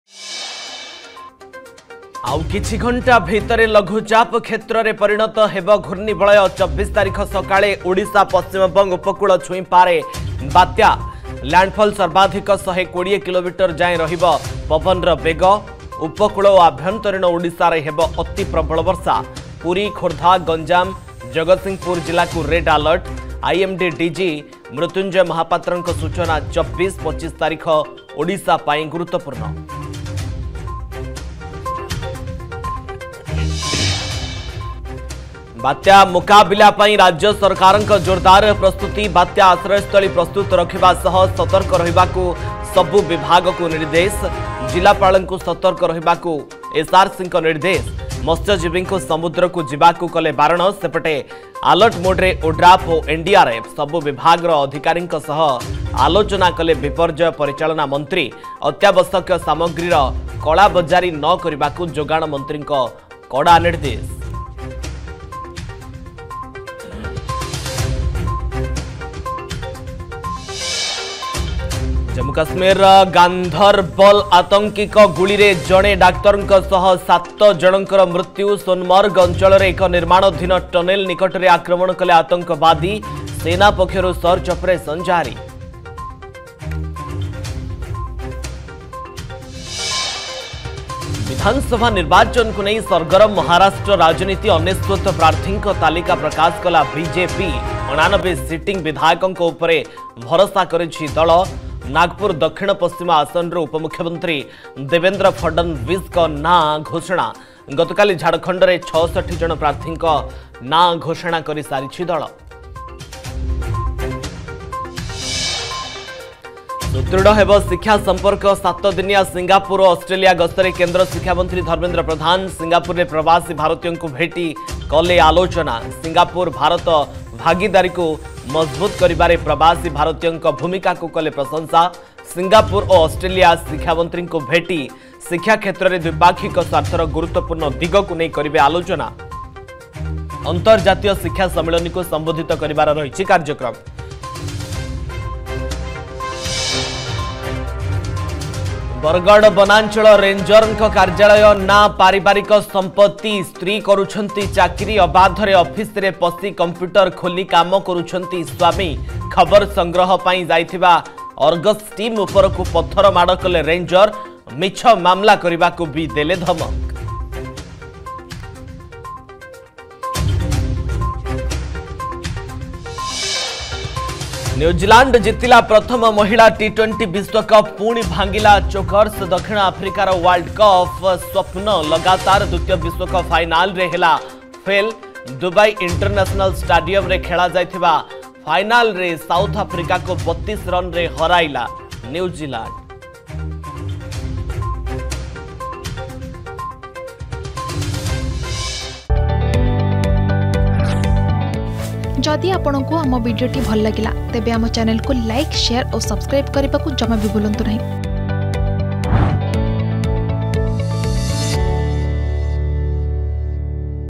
Headlines @ 7 AM || 21 Oct 2024 || Argus News
+ ବଳ ଗୋଟାଉଛି ବାତ୍ୟା
+ `ବାତ୍ୟା ମୁକାବିଲା - ସରକାର ପ୍ରସ୍ତୁତ'
+ ଆତଙ୍କୀଙ୍କ ଗୁଳିରେ ୭ ନିହତ